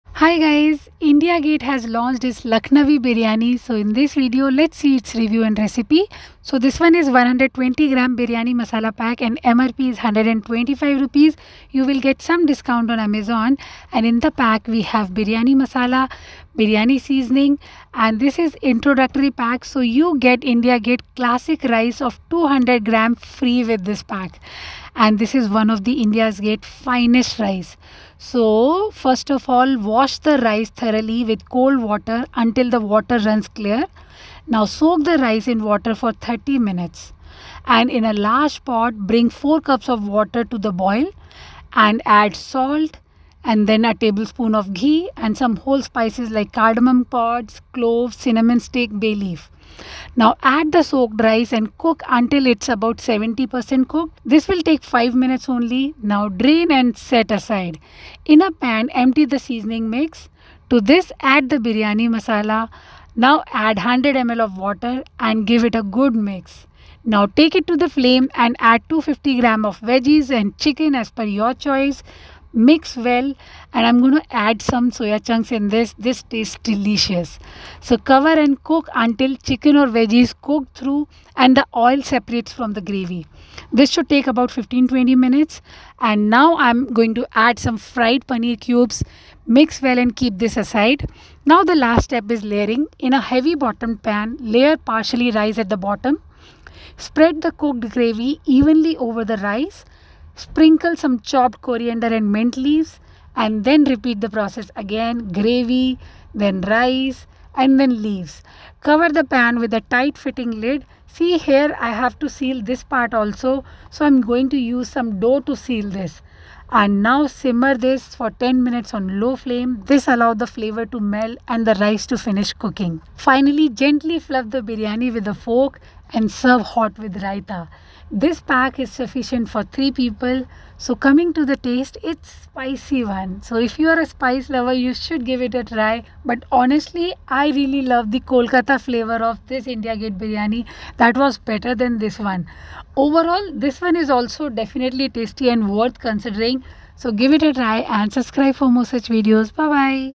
How to cook India Gate Lucknowi Biryani | India Gate Lucknowi Biryani Recipe & Taste Test
India Gate Lucknowi Classic Biryani Masala purchase